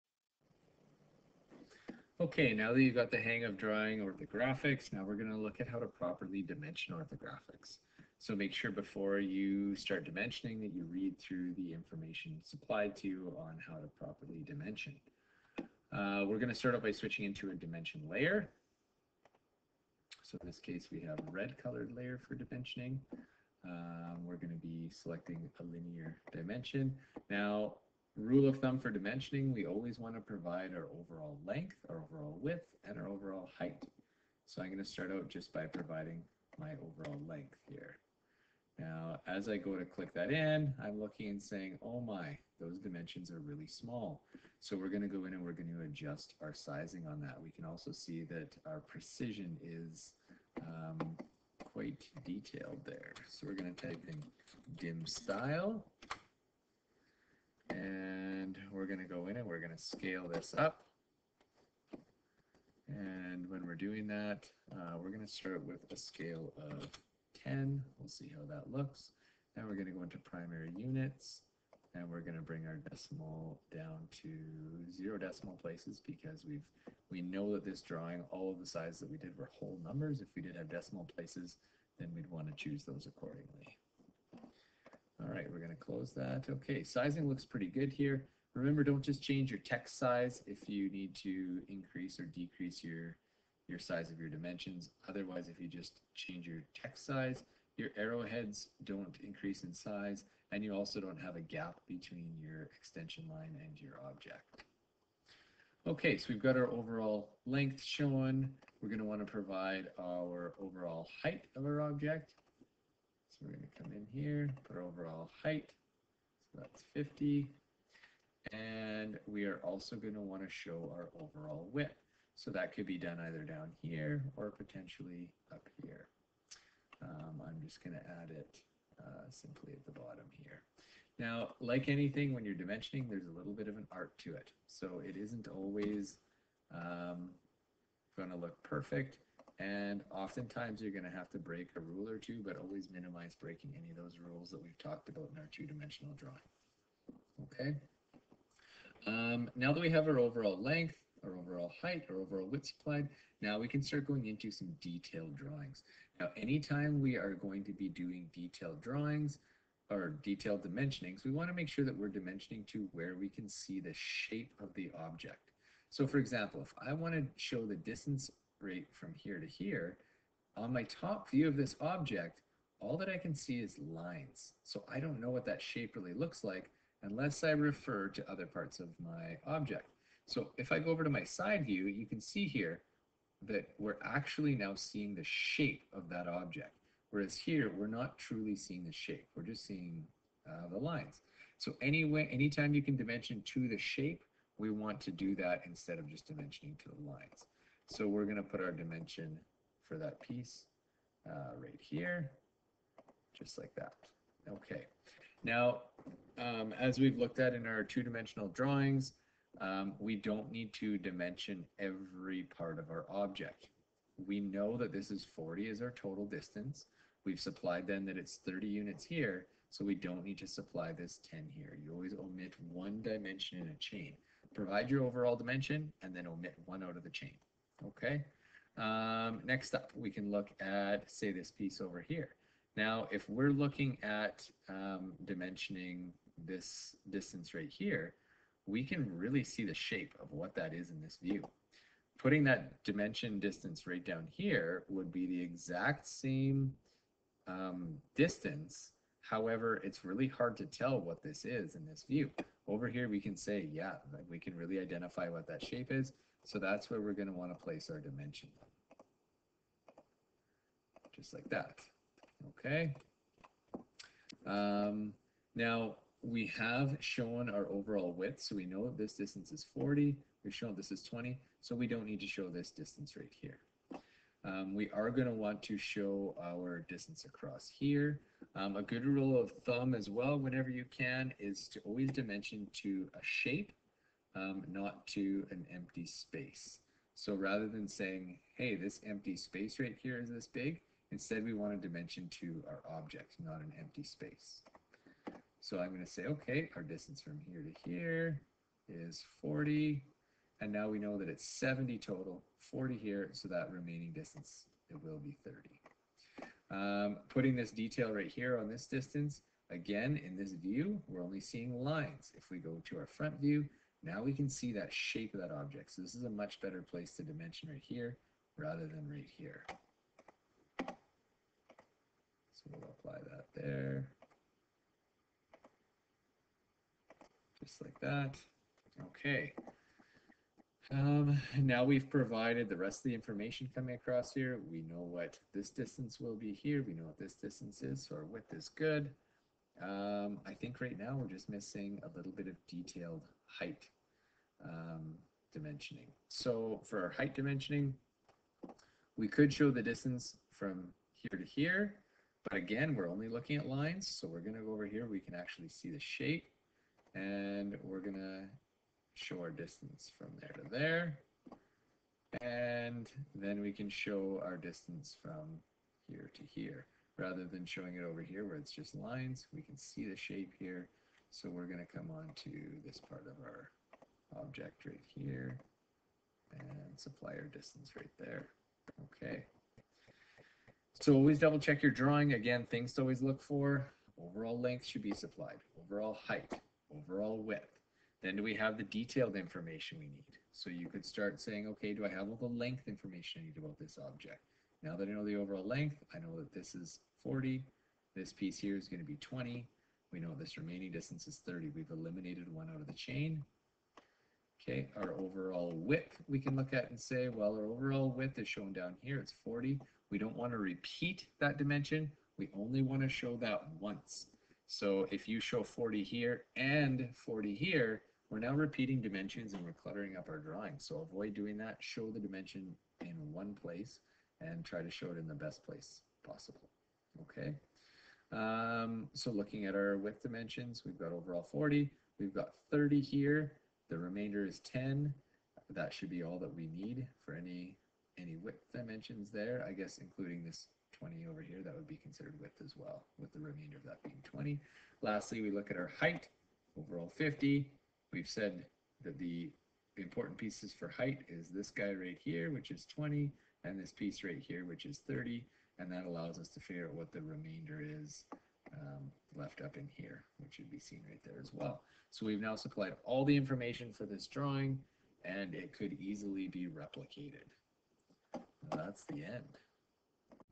How to Dimension an Orthographic Drawing in Auto-Cad
In this video I walk through the basic of applying dimensions to an orthographic drawing (multi-view).